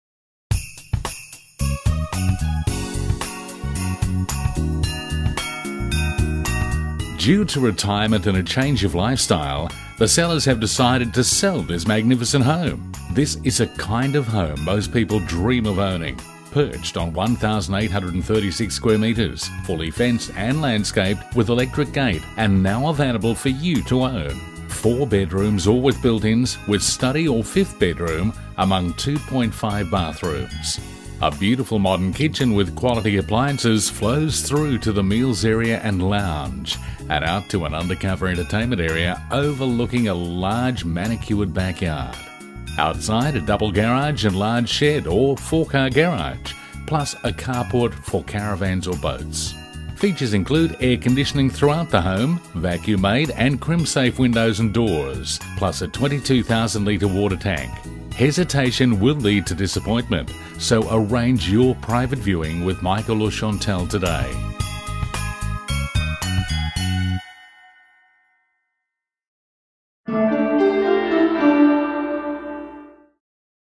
SOLD by MICHAEL VONDER RAY WHITE Aspley Real Estate and Property Agent BRACKEN RIDGE
SOLD SOLD SOLD SOLD 7 Rolls Royce Court, Joyner Marketed by Michael Vonder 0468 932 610 SOLD SOLD SOLD
Due to retirement and a change of lifestyle, the sellers have decided that it's now or never. Hesitation will lead to disappointment. This is the kind of home that most people dream of owning and now that opportunity has been made available to you for a very short time. It will be sold either before it goes to auction or under the hammer. Property of this calibre never last long on the market.

• Fully fenced and landscaped 1836m2 property with electric gate and 22,000 litre water tank.
• 4 or 5 bedrooms with study, 2.5 bathrooms, 2 garage and large shed or 4 garage. Carport for caravan or boat.
• Kitchen, dining and passage areas are tiled, bedrooms and lounge carpeted.
• Beautiful kitchen with quality appliances, air conditioning throughout the home, Vacuum maid and Crimsafe.
• Lots of storage space throughout the home with a spacious laundry and built-in cupboards in all bedrooms.
Call Michael today on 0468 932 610 and make a time to inspect this lovely family home.